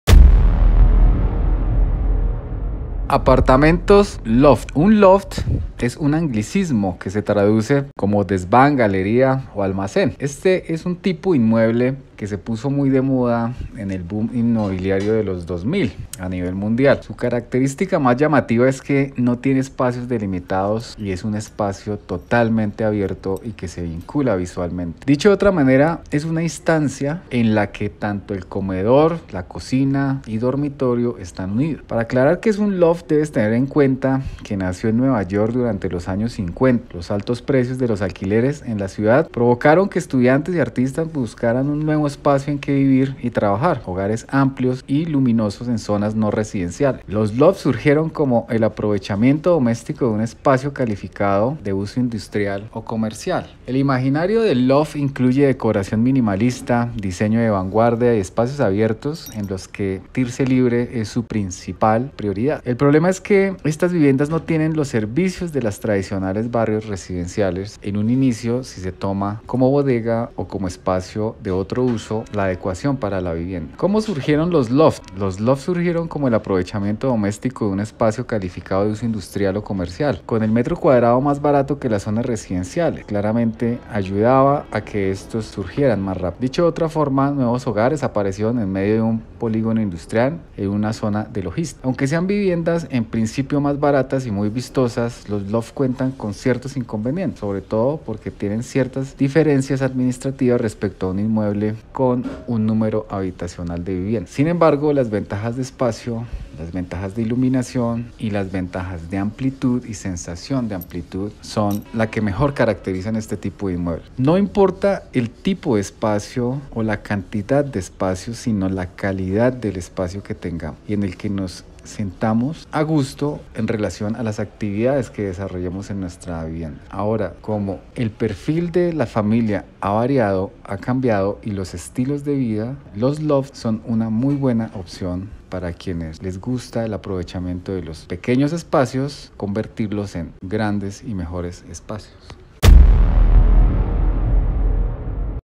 Apartamento loft
En el presente video describimos que es un loft.
Hemos diseñado espacios loft bajo parámetros específicos, los más retadores han sido los de metrajes muy bajos.